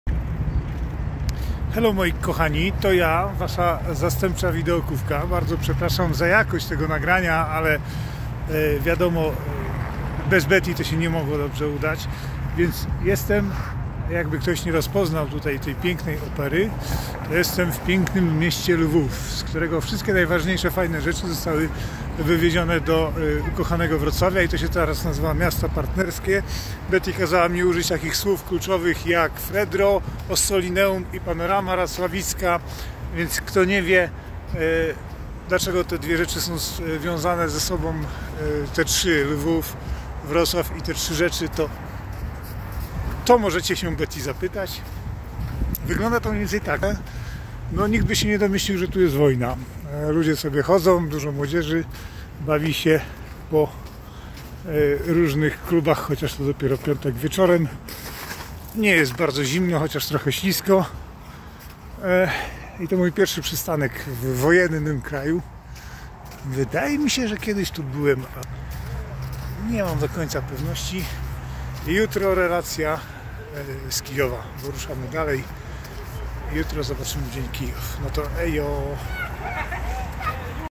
Noc we Lwowie
Pan Videokówka jest w Ukrainie, skąd - jeśli mu obowiązki pozwolą - będzie nas czasem pozdrawiał